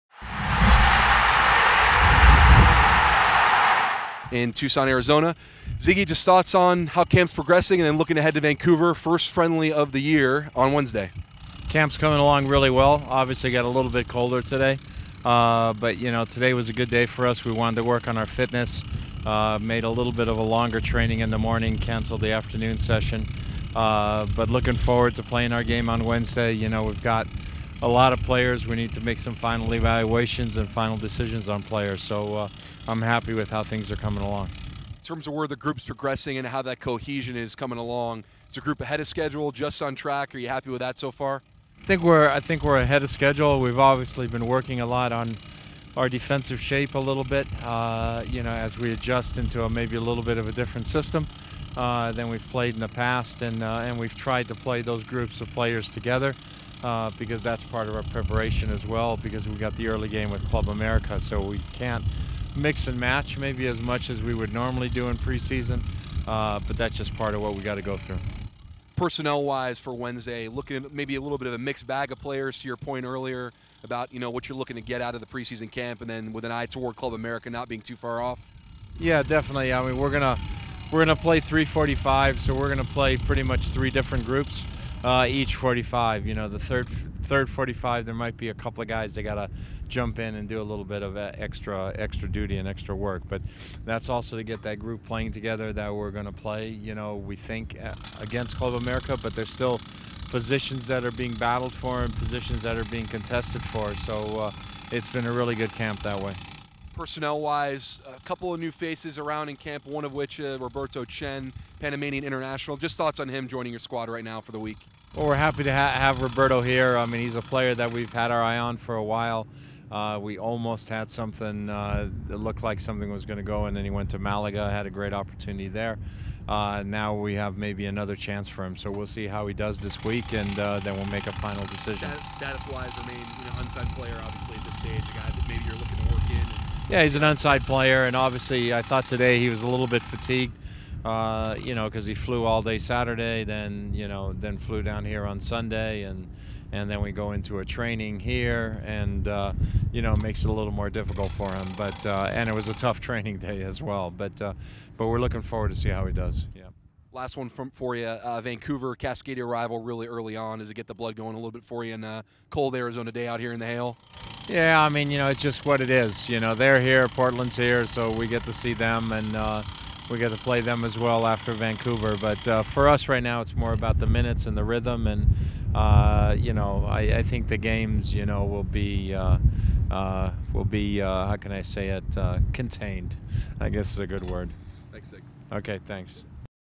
Interview: Sigi Schmid on Roberto Chen trialing with the club
Seattle Sounders FC Head Coach Sigi Schmid speaks to SoundersFC.com on the upcoming preseason friendly vs Vancouver Whitecaps FC, Roberto Chen's trial with the club and the battle for roster spots in week two of camp.